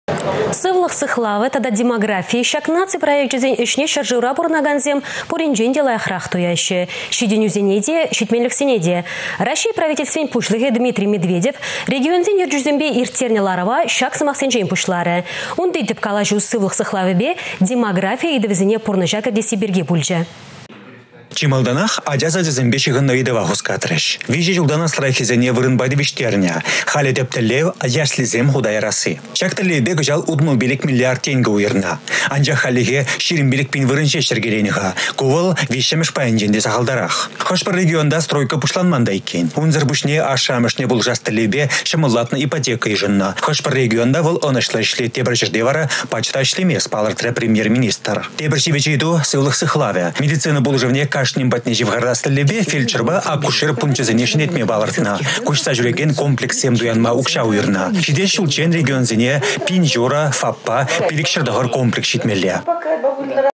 Демографи тата сывлăх сыхлавĕ
Сывлăх сыхлавĕ тата демографи — çак наци проекчĕсен ĕçне çĕршывра пурăнакансем пуринчен те лайăхрах туяççĕ: çитĕнӳсене те, çитменлĕхсене те. Раççей правительствин пуçлăхĕ Дмитрий Медведев регионсен ертӳçисемпе ирттернĕ ларăва çак сăмахсенчен пуçларĕ. Унти тĕп калаçу сывлăх  сыхлавĕпе демографи ыйтăвĕсене пурнăçа кĕртесси пирки пулчĕ.